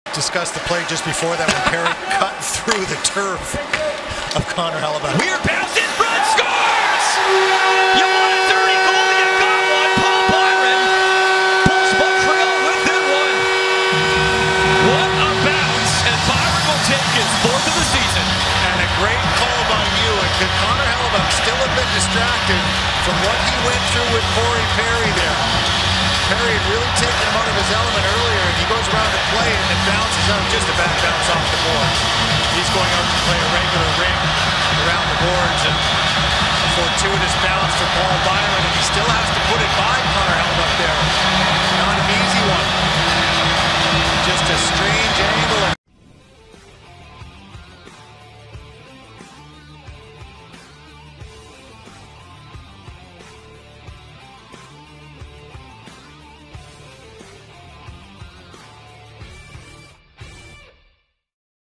Connor Hellebuyck Frustrated After Unlucky Bounce Sets Up Open Net For Paul Byron
Connor Hellebuyck showed some frustration after an unlucky bounce allowed Paul Byron to score on a wide open net.